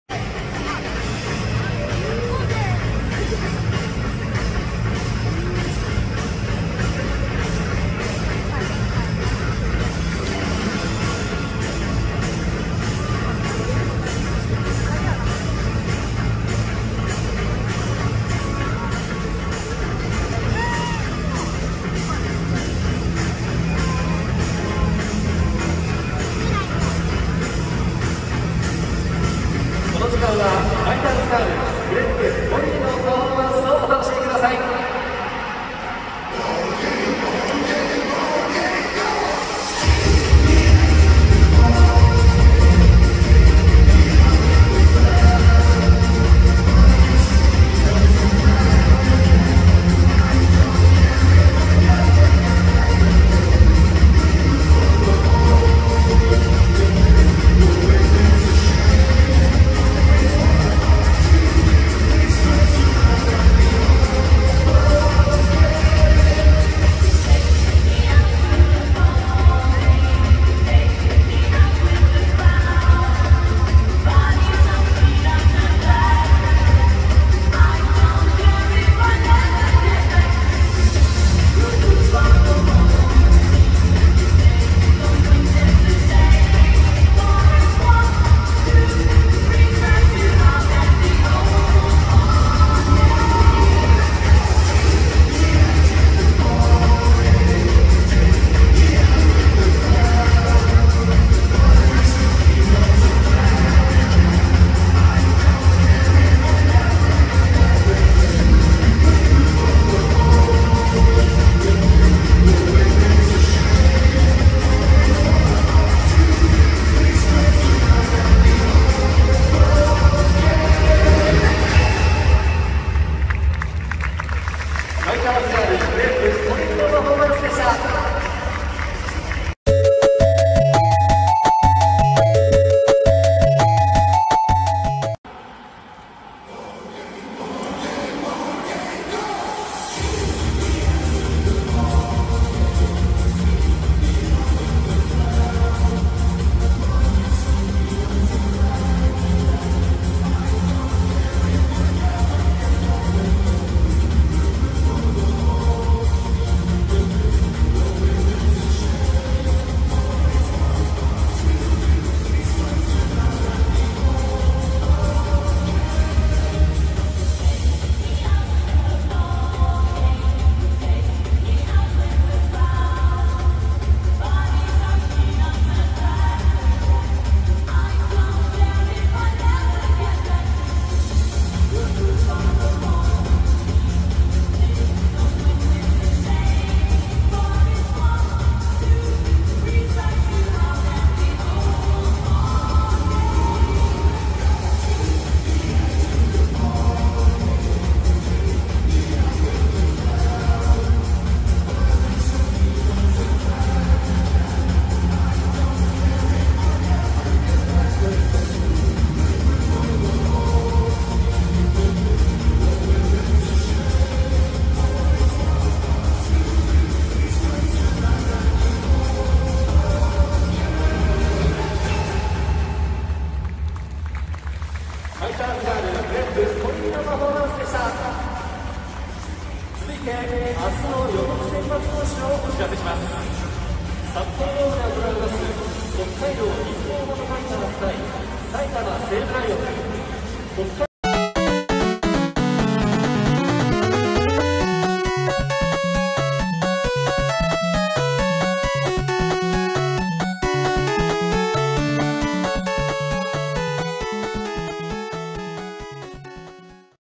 2020/9/3 ファイターズガールパフォーマンス
札幌ドームで行われた東北楽天ゴールデンイーグルス戦での、ファイターズガールのダンスシーンです。